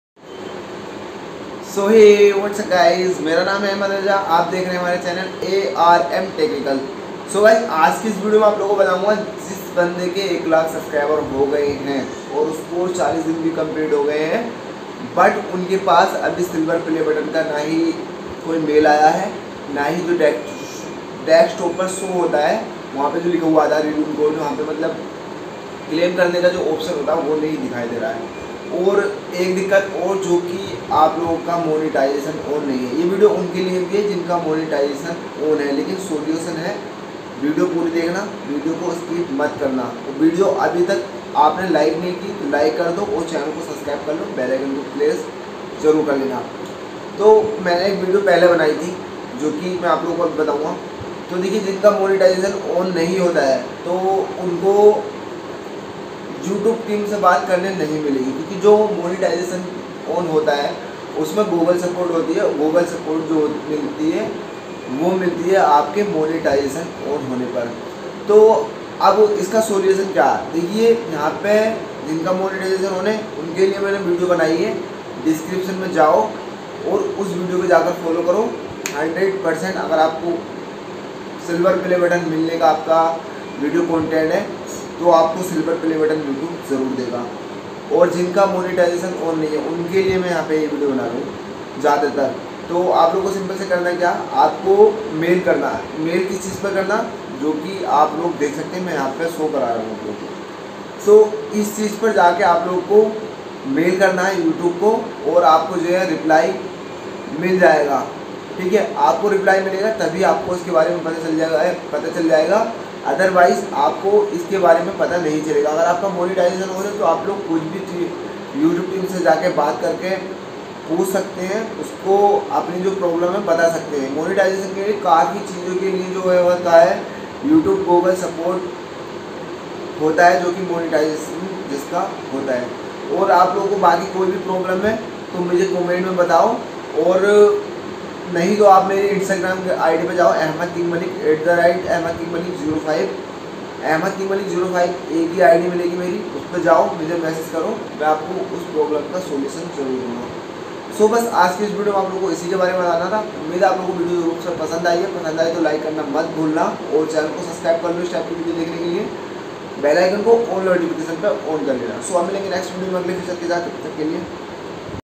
100k subscribe complete but Silver Play button claim mail nahi aaya || monetization not on solution
कोई भी परेशानी हो तो कमेंट कर सकते हो नही तो मेरे इंस्टाग्राम का नाम @ahmadkingmalik05 पर जाओ और फोटो दो परेशानी बताओ

Instagram.com/ahmadkingmalik05